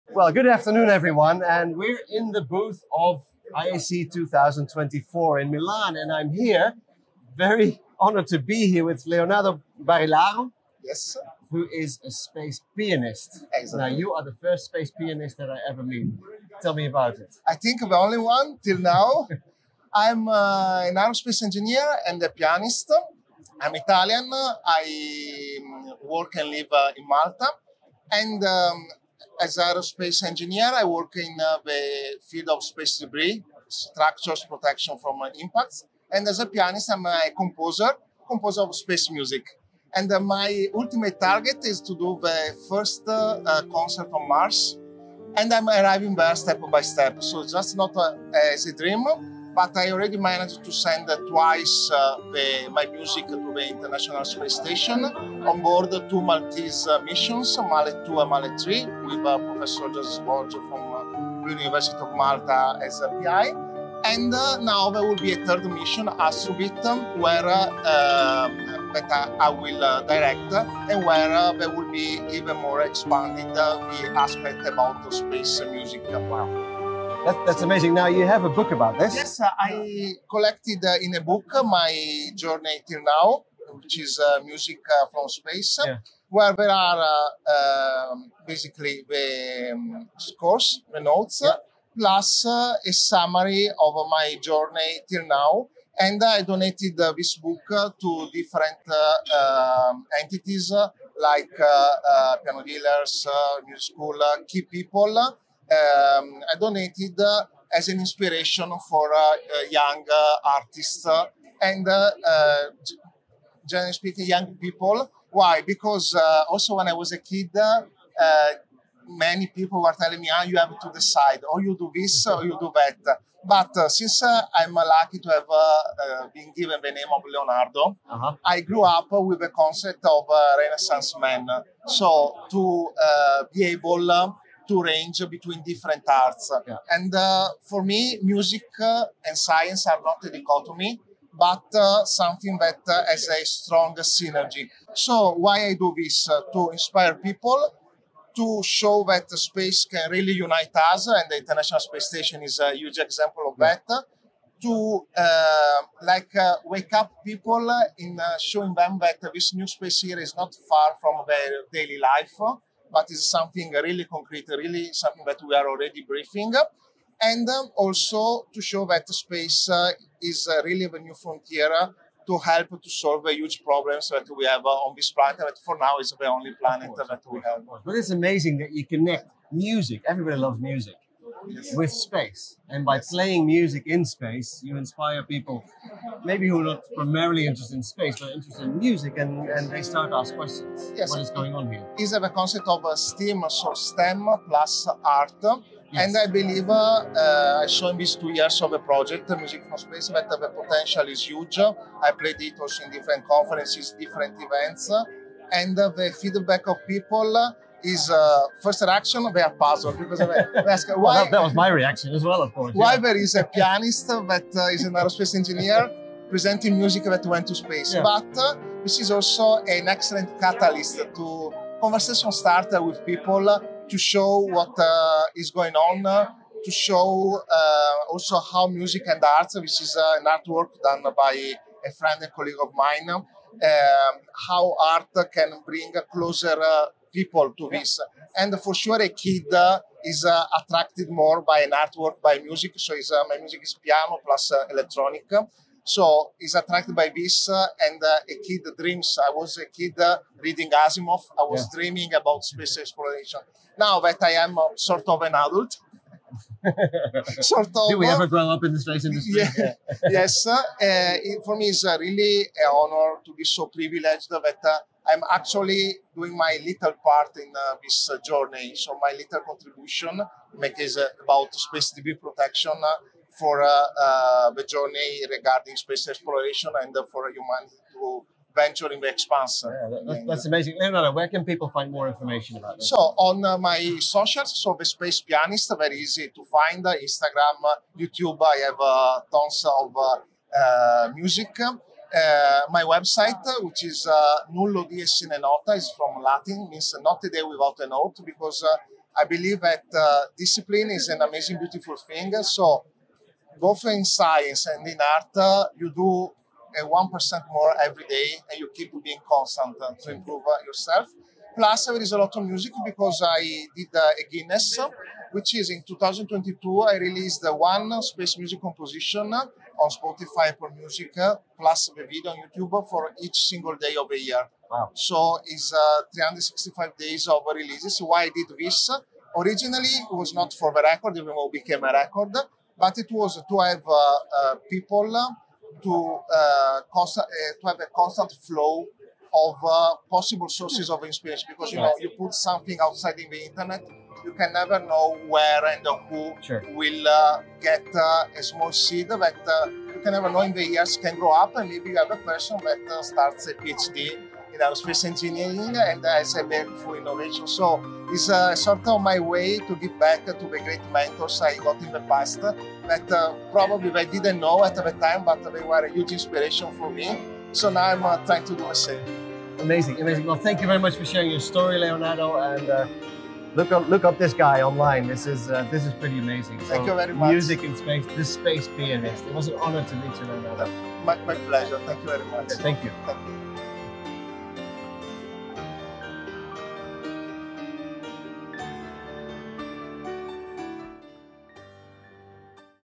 Interview to Leonardo the Space Pianist by Remco Timmermans at IAC 2023
A beautiful interview by Mr Remco Timmermans, Social Media Specialist for Space. We talked about my project Music from Space and what is next.
I was really happy to meet him in the busy space-hive of IAC 2023.
I discovered a very nice and kind person, a great public speaker and with a rare insight ability in the complex space domain.

IAC 2023 - Give Space a Chance - 74th International Astronautical Congress in Baku, Azerbaijan. What an event! Huge conference with more than 5000 people attending. The state of the art of the new frontiers in Space.

Leonardo Barilaro is a pianist, composer and aerospace engineer. And he will play the first piano concert on Mars.
Through his innovative fusion of music and science, Leonardo the Space Pianist aims to inspire and spread awareness about Space exploration and its possibilities for Human civilization.
He thinks that music can transcend cultural barriers and connect people from all over the world to the wonders of Space. Leonardo also promotes the idea of sustainability and environmental awareness, as he believes that the exploration and colonization of Space can help mitigate the impact of the climate crisis on Earth.

Video courtesy by Remco Timmermans